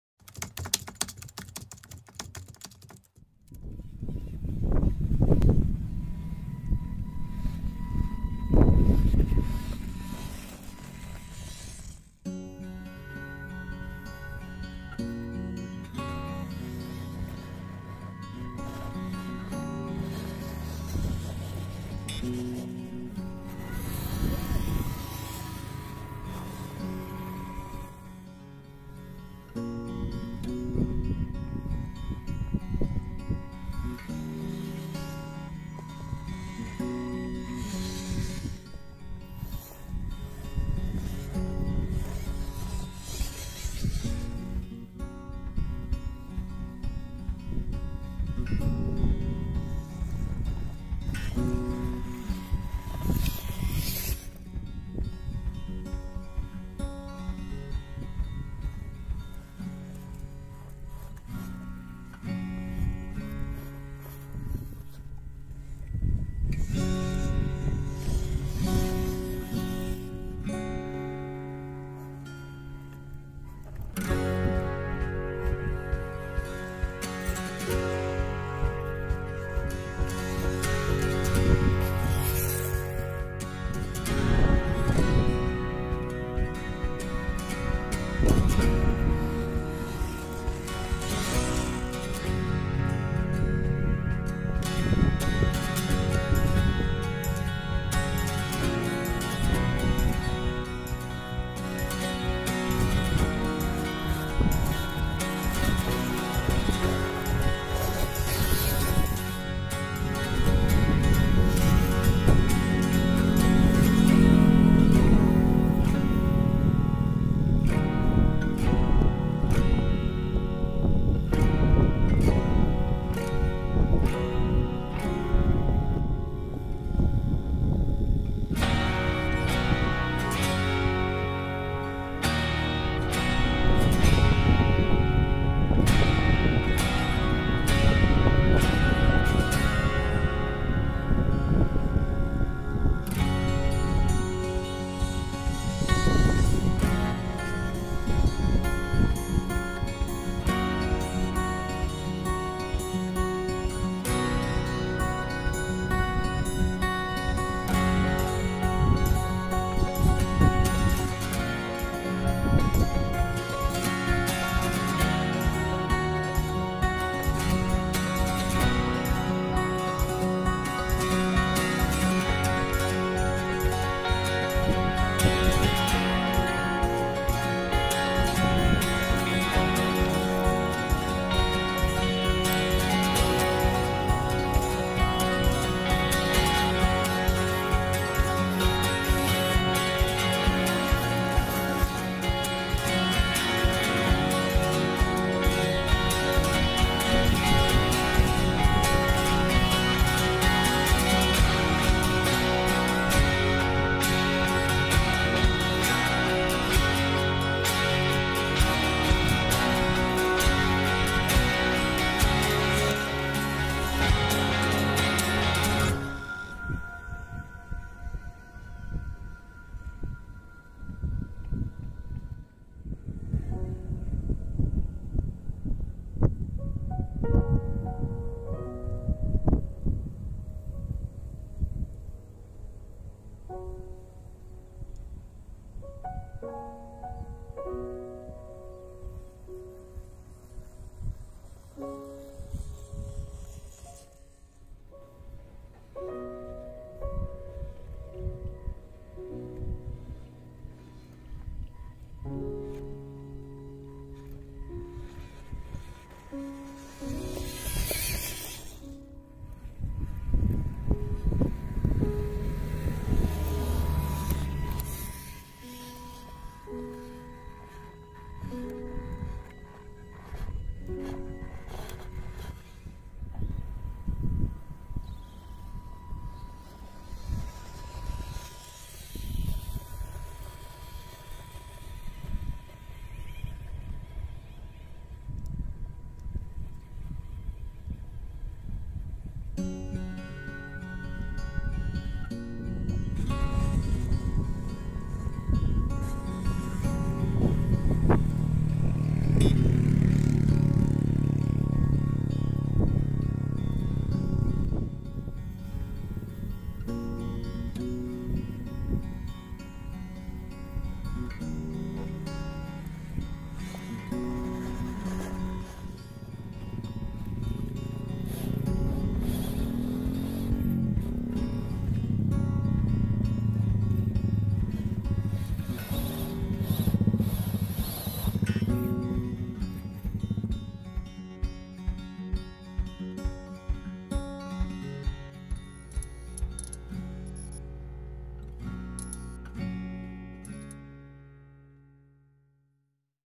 Rc Car MN-D90 "construction area trail"
Halo Rc lovers, track lumayan berat sampai akhirnya motor mati 🤣